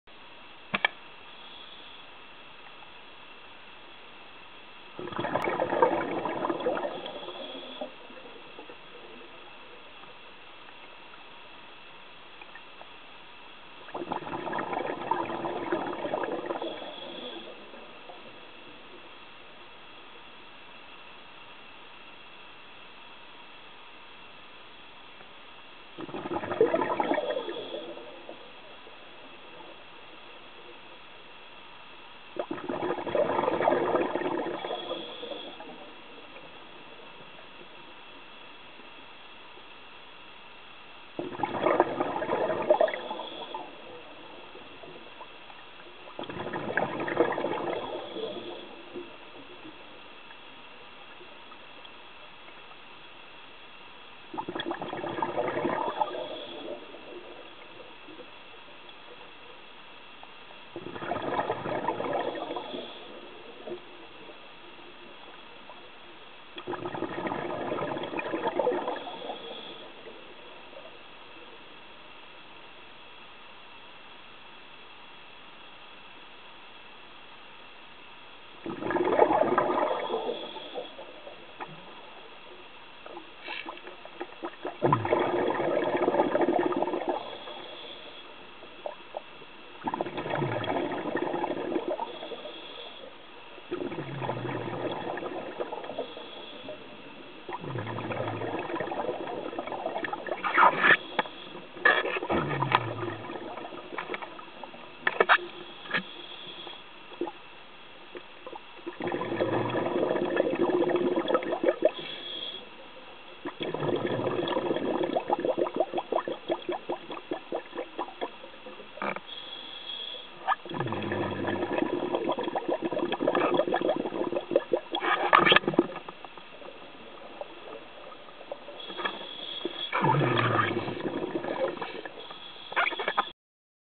Plankton soup
Short clip taken with compact camera (Nikon P6000)  just to record ~1,5 m thick surface water layer full of copepods and plankton. It clearly differentiated from the rest of the water column while seen from down below. Agust 2014, Isfjorden, Spitsbergen.